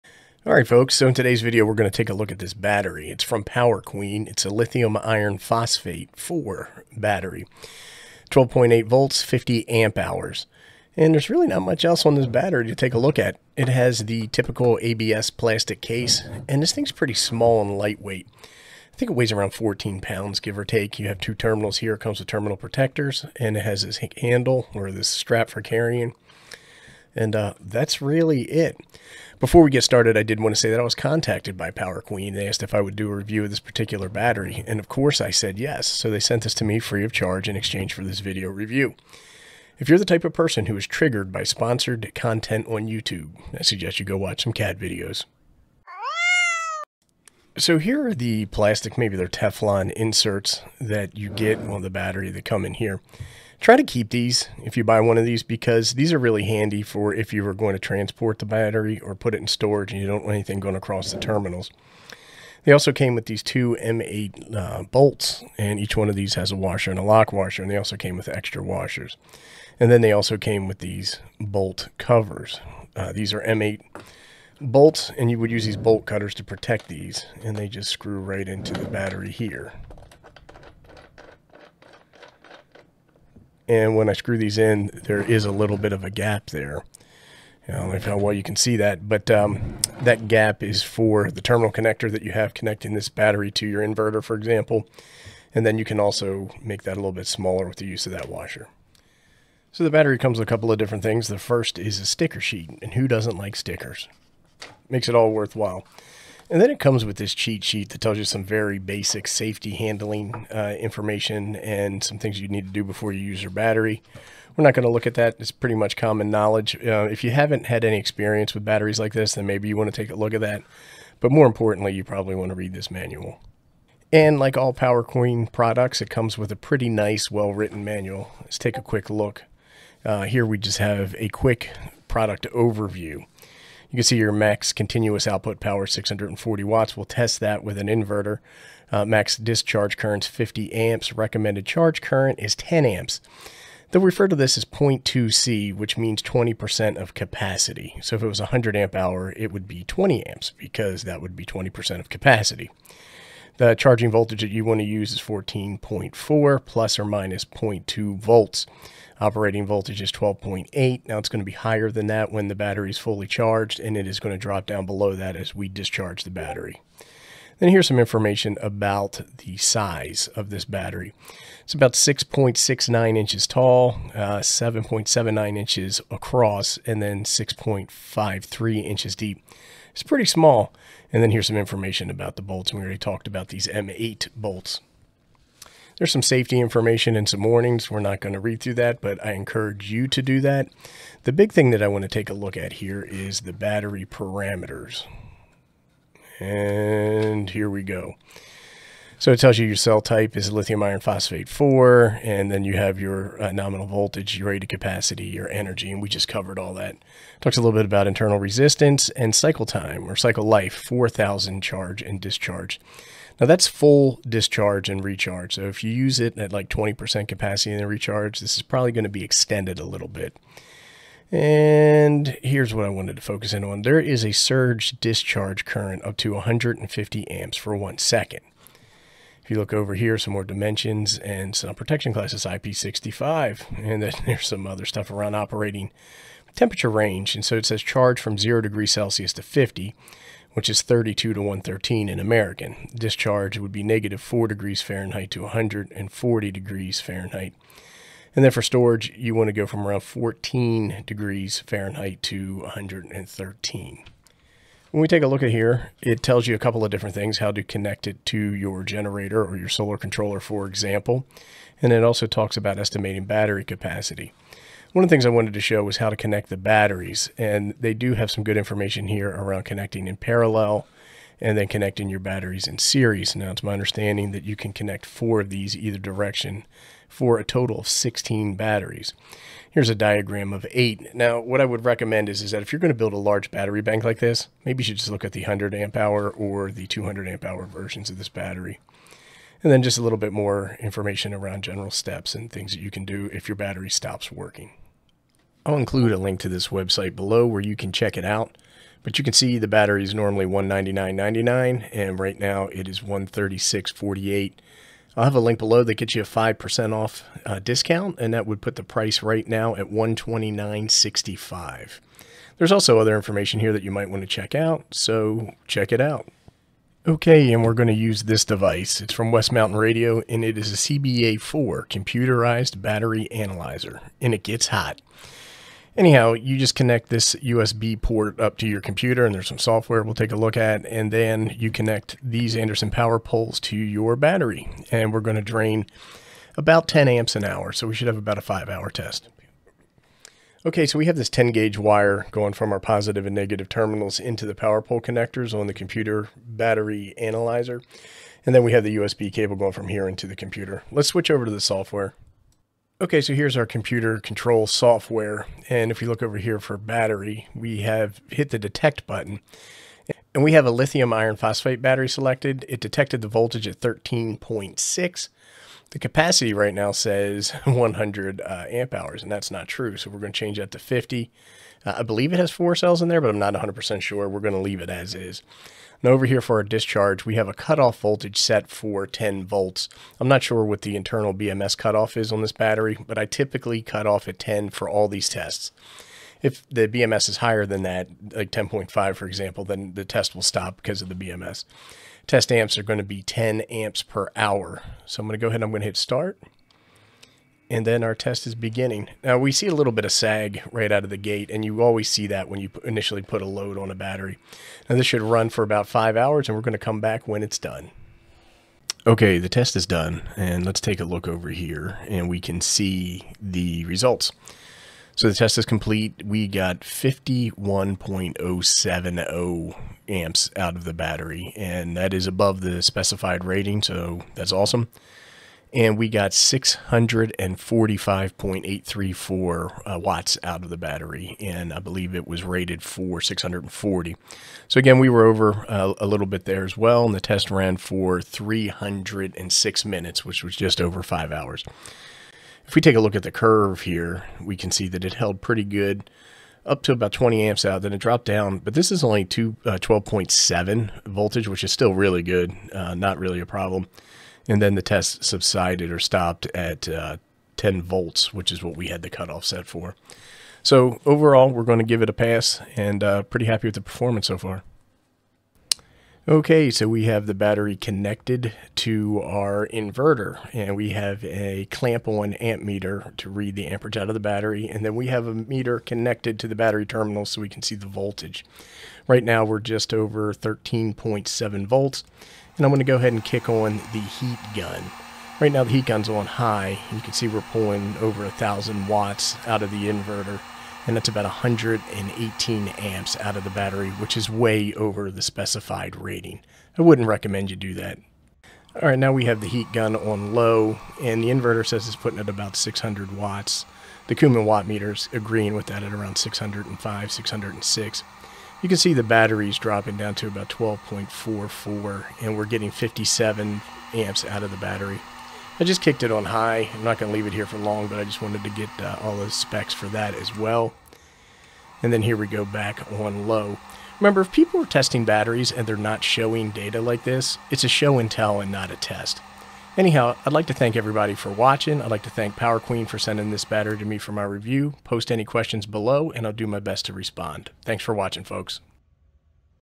Power Queen 50Ah LiFePO4 Battery: Does it PACK a Powerful PUNCH?
In this video, we dive into testing the power queen 50Ah 12.8V LiFePO4 battery to see if it truly packs a powerful punch. Join us as we explore the performance and capabilities of this high-capacity battery in real-world scenarios. If you're considering investing in a reliable power source for your devices or projects, this review will provide you with valuable insights. Watch till the end to discover if this battery is the right choice for your power needs!

Note: This battery was sent to me free of charge in exchange for this video review.

Links to products in the video:
👉Kuman Watt Meter